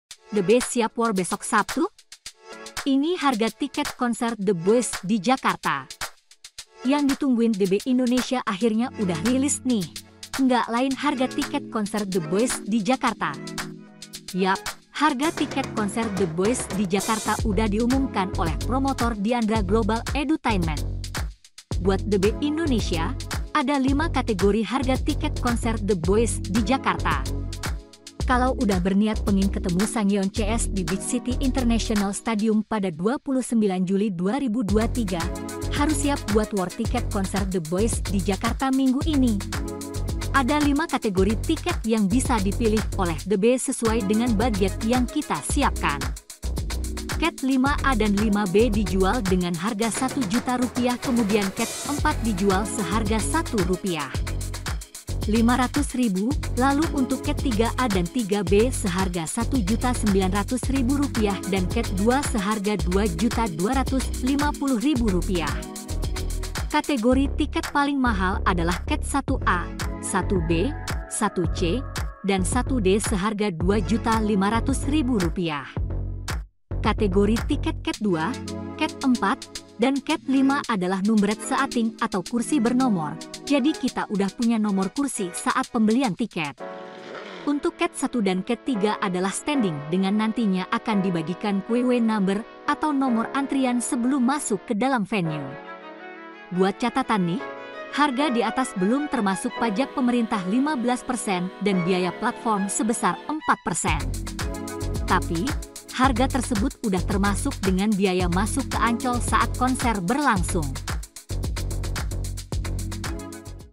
TheB Siap War Besok Sabtu Ini Harga Tiket Konser THE BOYZ di Jakarta
CewekBanget.ID - Yang ditungguin TheB Indonesia akhirnya udah rilis nih, enggak lain harga tiket konser THE BOYZ di Jakarta!

Yup, harga tiket konser THE BOYZ di Jakarta udah diumumkan oleh promotor Dyandra Global Edutainment.

Buat TheB Indonesia, ada 5 kategori harga tiket konser THE BOYZ di Jakarta.

Kalau udah berniat pengin ketemu Sangyeon cs di Beach City International Stadium pada 29 Juli 2023, harus siap buat war tiket konser THE BOYZ di Jakarta minggu ini!

Selengkapnya, yuk kepoin info tentang harga tiket konser THE BOYZ di Jakarta yang bertajuk THE BOYZ 2ND WORLD TOUR : ZENERATION ini.